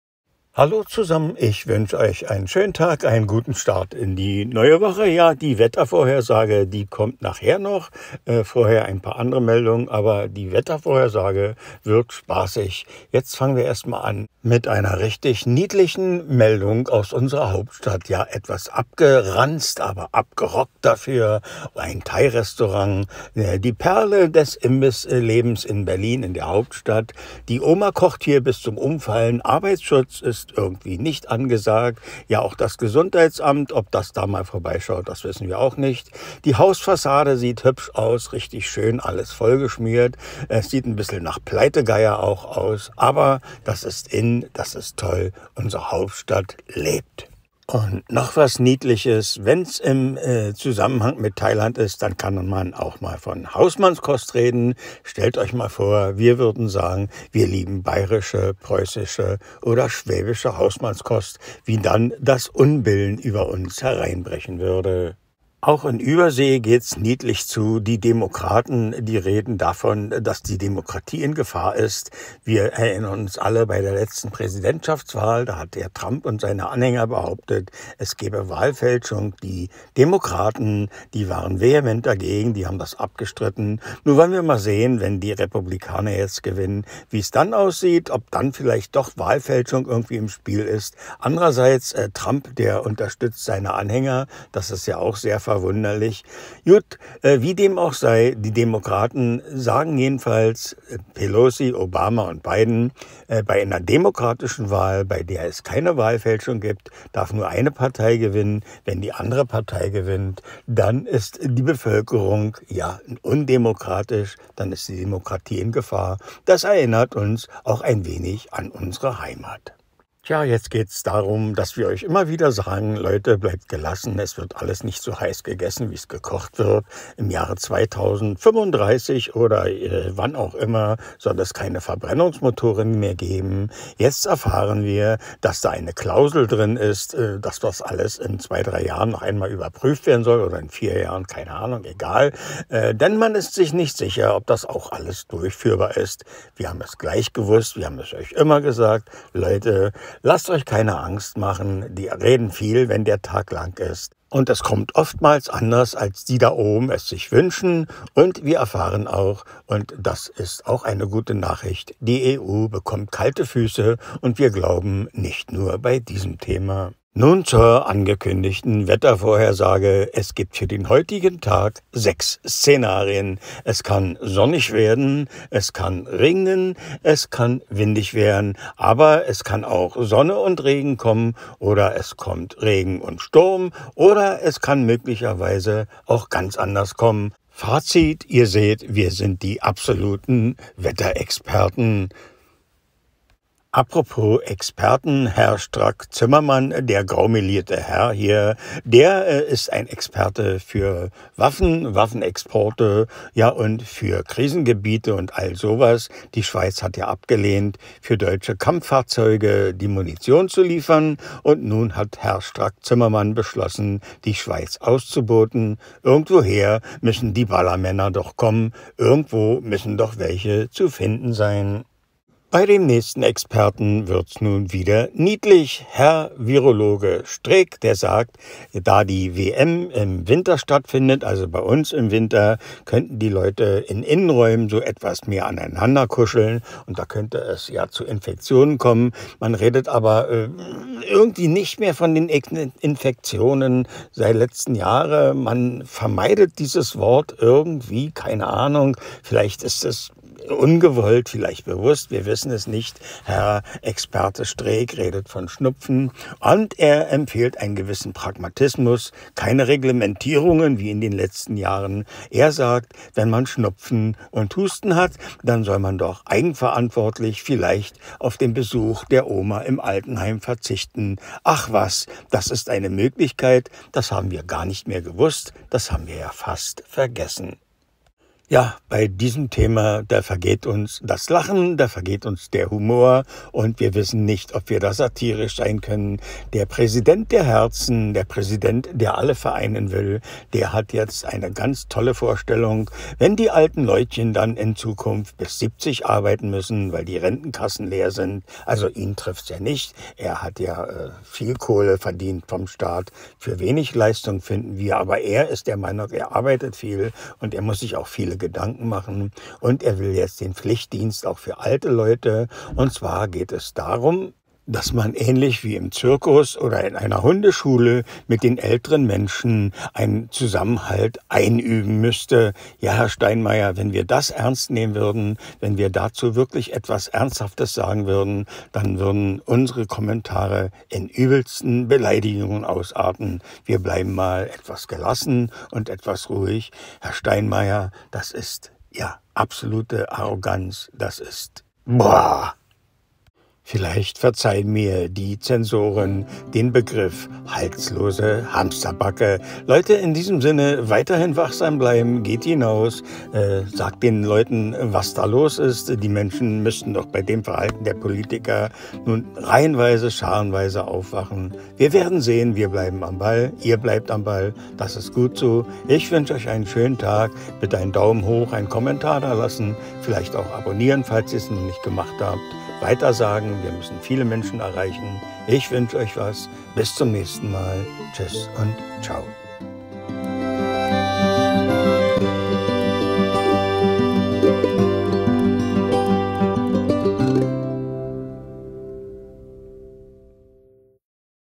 Bundespräsident Steinmeier - Unfassbare Tragödie - Unglaubliche Meldung
Steini übt mit Rentnern Künststückchen ein….!
USA werden zur Diktatur….!

Mackys Meinung

Danke